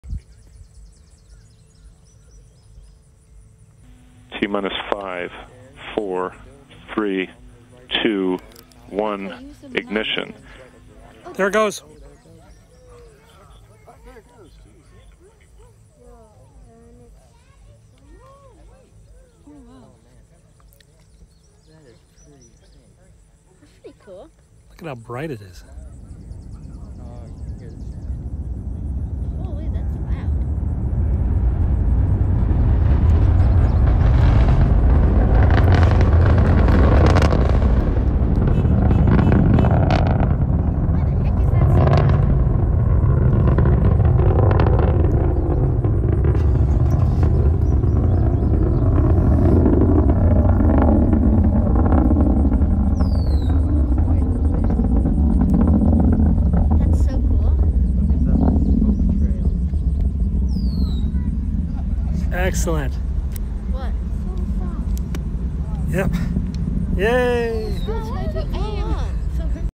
Camera 3 of NASA Minotaur IV launch Wallops Island with NROL-129 Spy Satellites - July 15, 2020
My iPhone's footage of the Minotaur IV launch from Wallops Island, zoomed it to the max and filmed with a Moment anamorphic lens. Unfortunately, the camera fell off the tripod in the middle and had to be readjusted quickly. Still quite fun.a